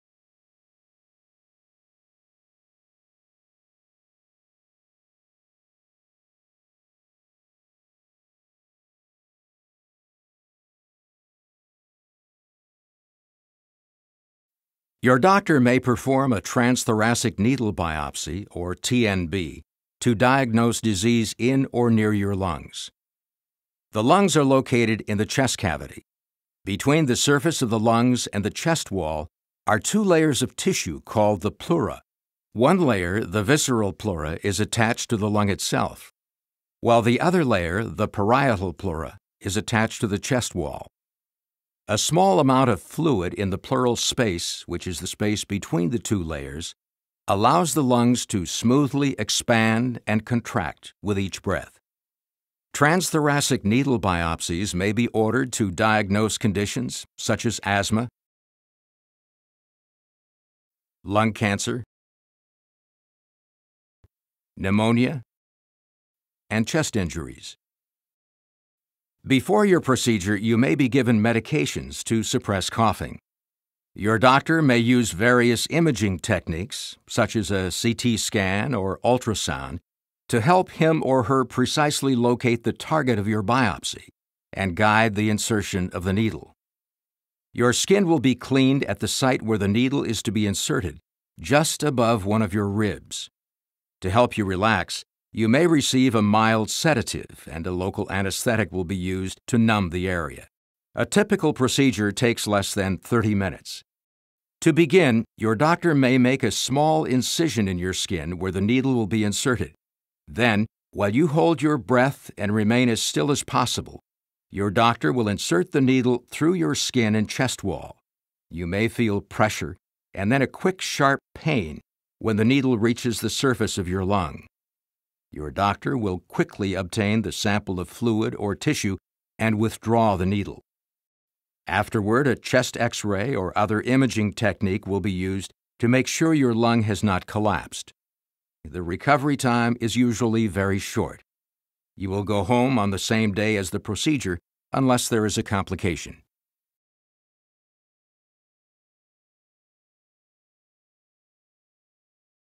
Lung Biopsy (TNB)
This 3D animation of a transthoracic needle biopsy, or TNB, a needle is used to obtain a small sample of fluid or tissue in or near the lungs to test for asthma, lung cancer, pneumonia, or chest injuries.
ANH00018